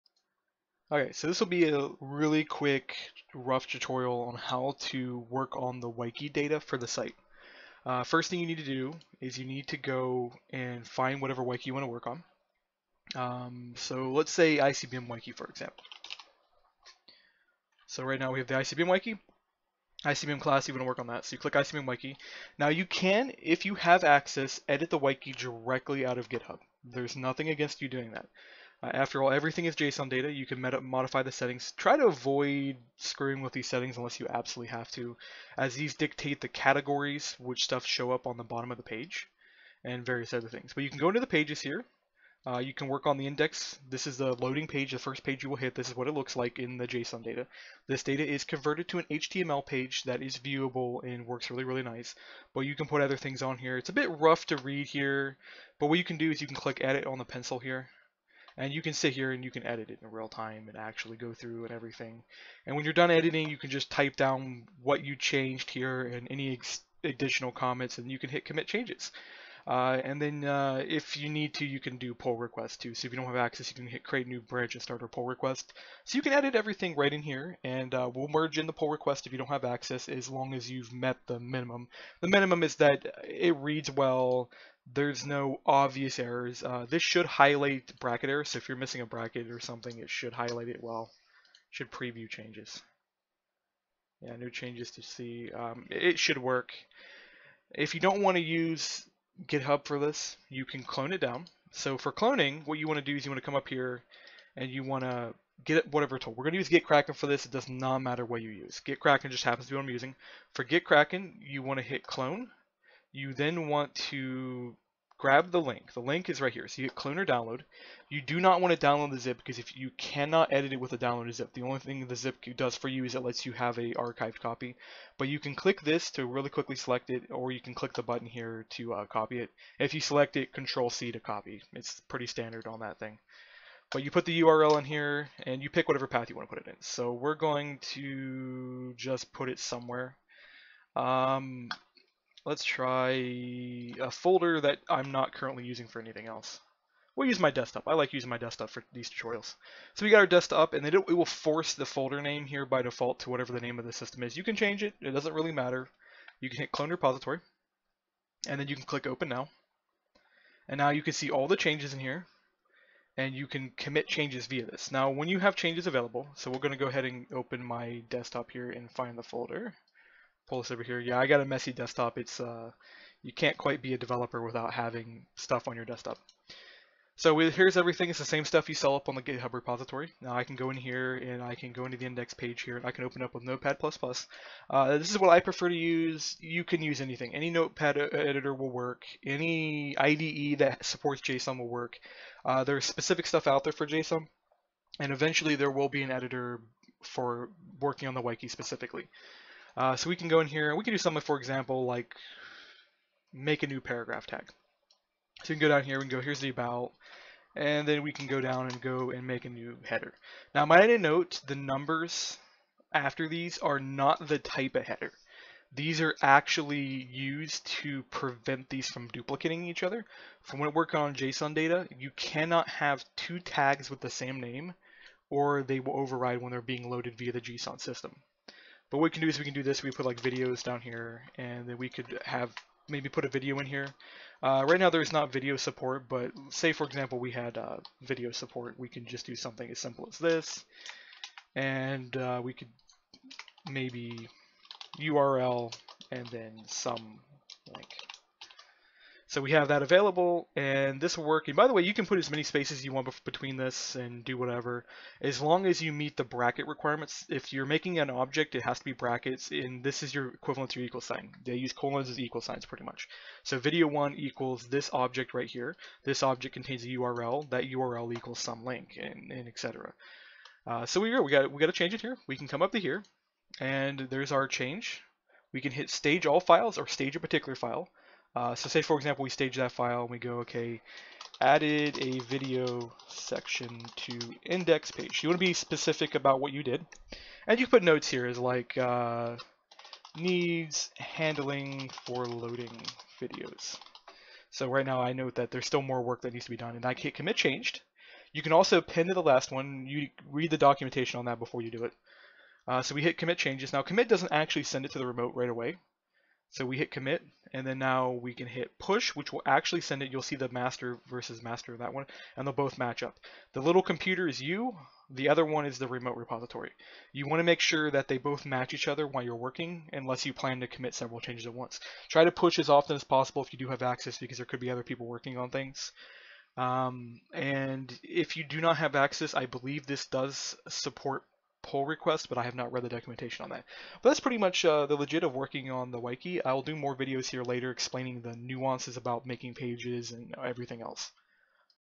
Wiki Tutorial #1 - Getting started
Quick rough draft tutorial on how to get started with working on the BBM wiki system.

IRC @Espert